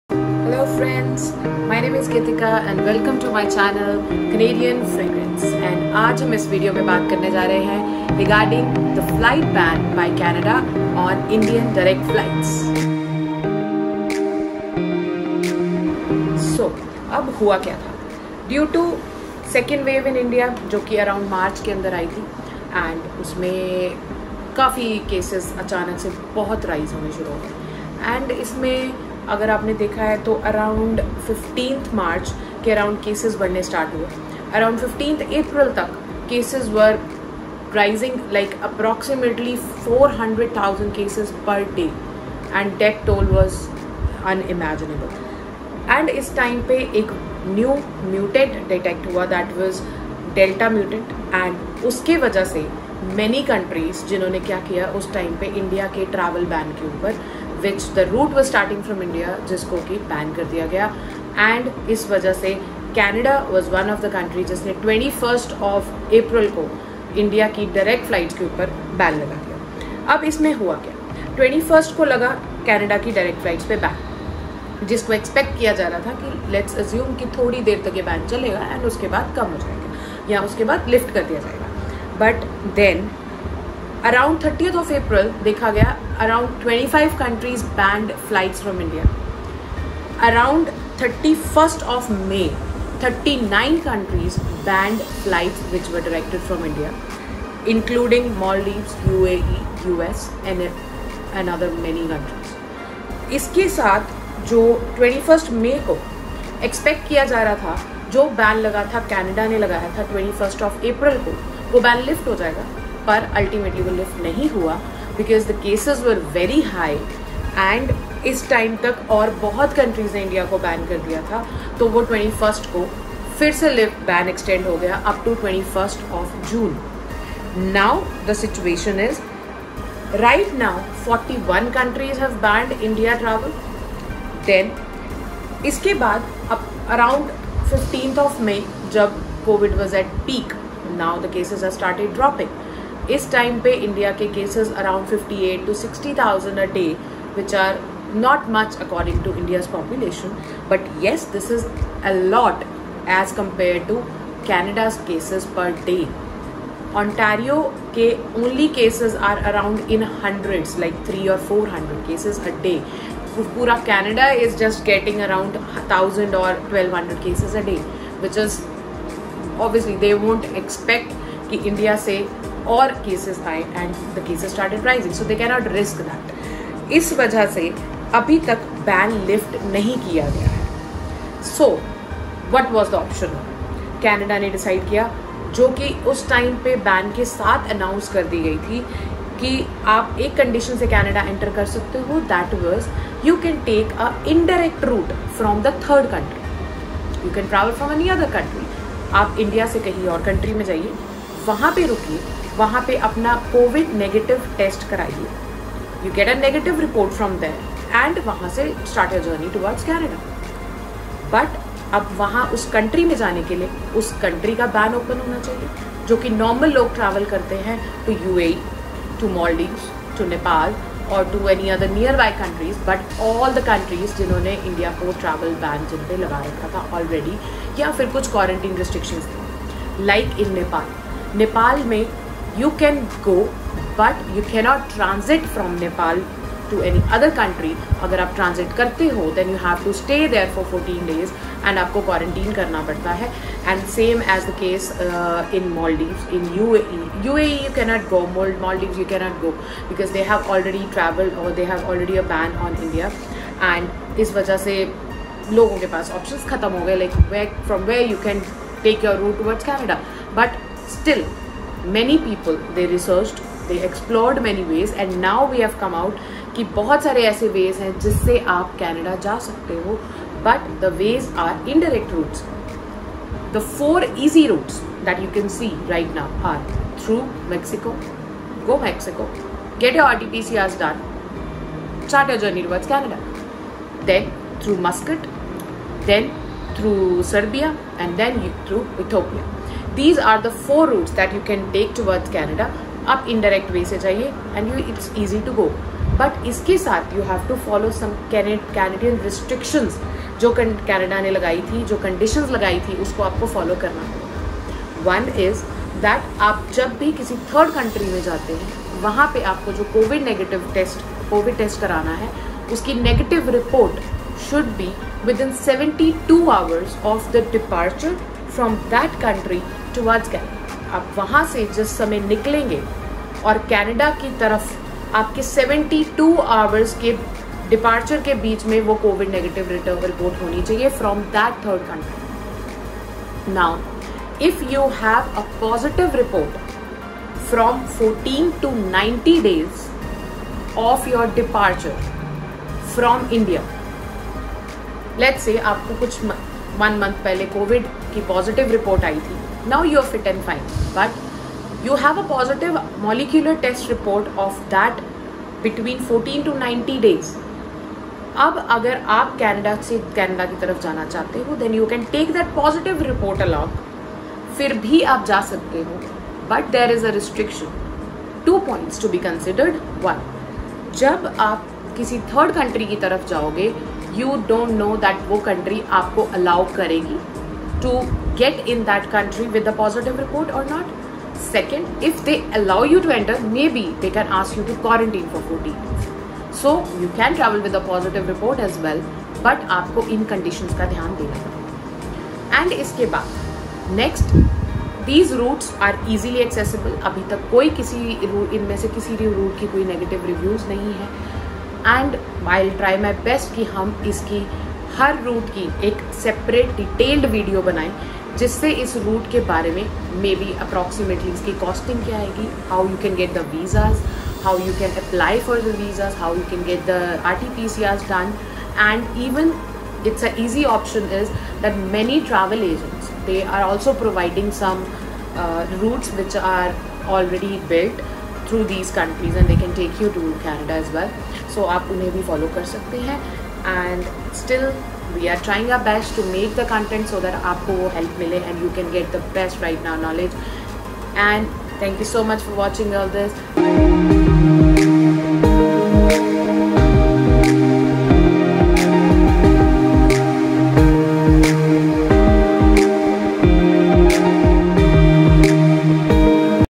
Direct Flights Ban from India to Canada | Travel Restrictions
Direct Flights Ban from India to Canada.

Reach us More

If you like to sponsor a video please contact as follows